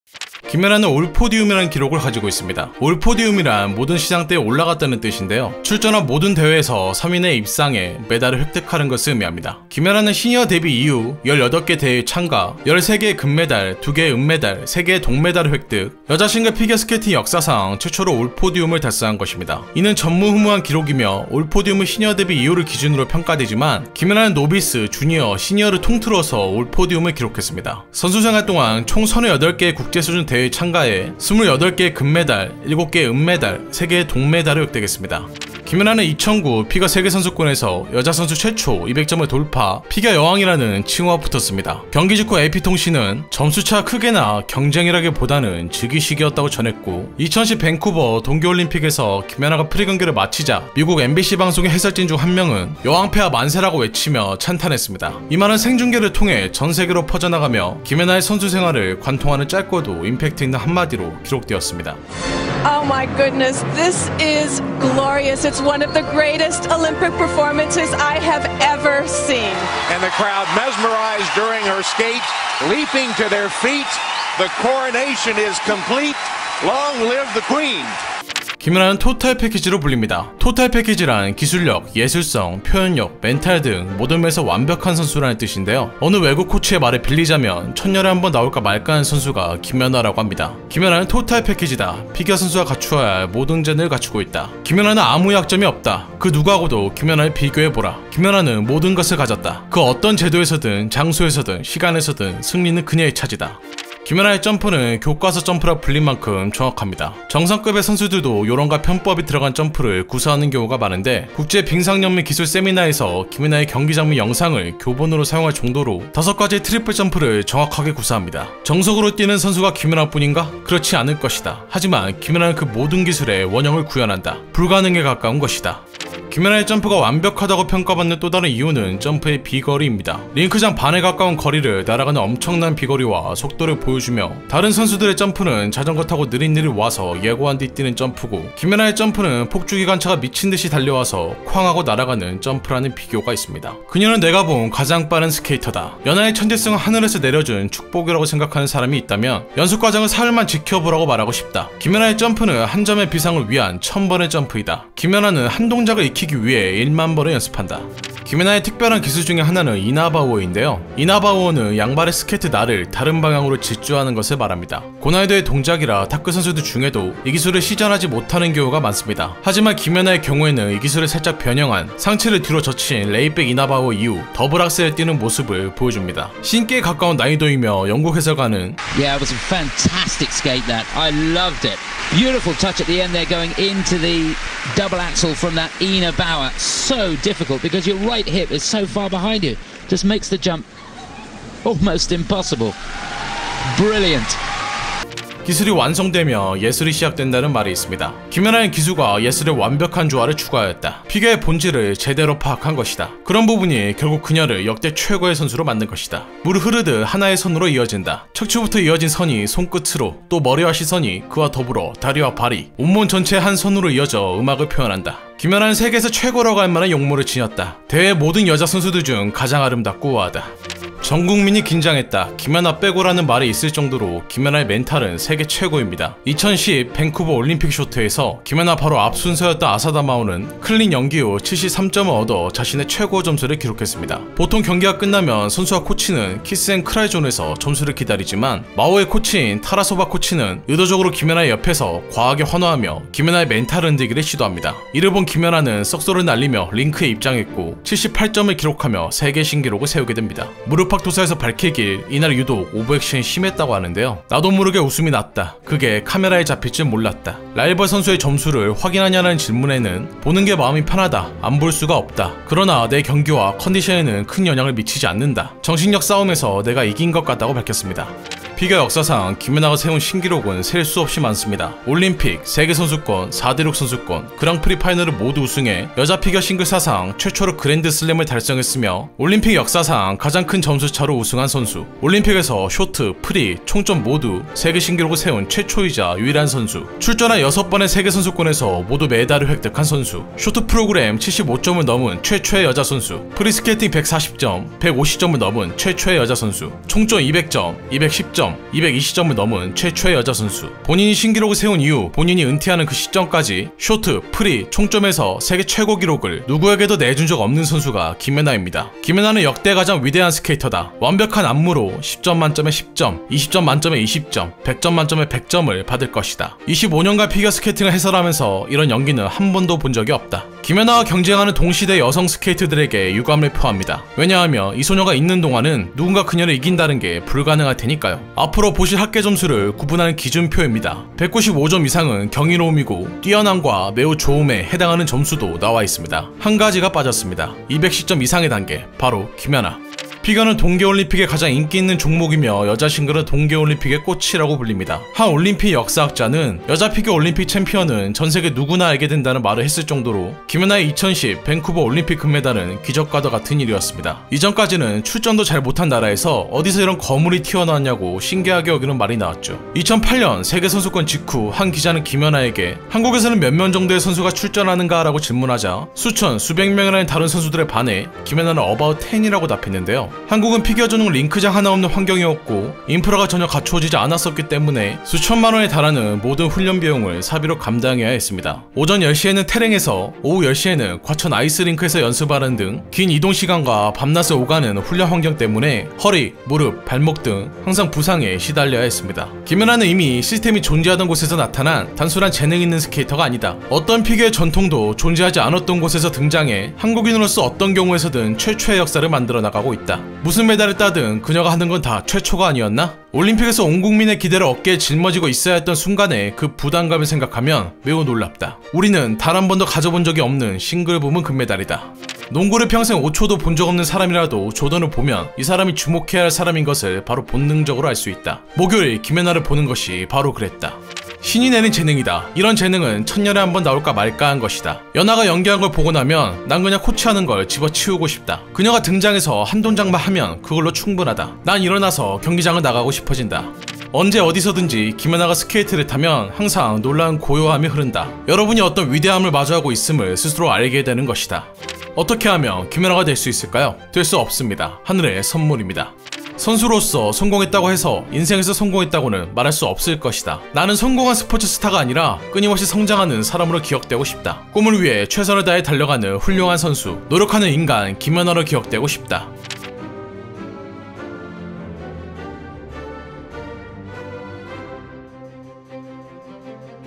국뽕 999%
피겨여왕 김연아에 대한 모든것
1. 올포디움
2. 피겨 여왕, 밴쿠버 올림픽
3. 토탈 패키지
4. 교과서 점프
5. 비거리
6. 이나바우어
7. 예술성
8. 강철 멘탈
9. 기록의 김연아
10. 피겨 불모지
11. 전문가들의 평가, 김연아 가라사대

유튜브 공유 기능 외 무단 업로드(페이스북) 금지합니다.

세상의 모든 인물, 인물백과사전입니다
더 많은 인물에 대해 알고 싶다면,
인물백과를 구독하고 받아보세요!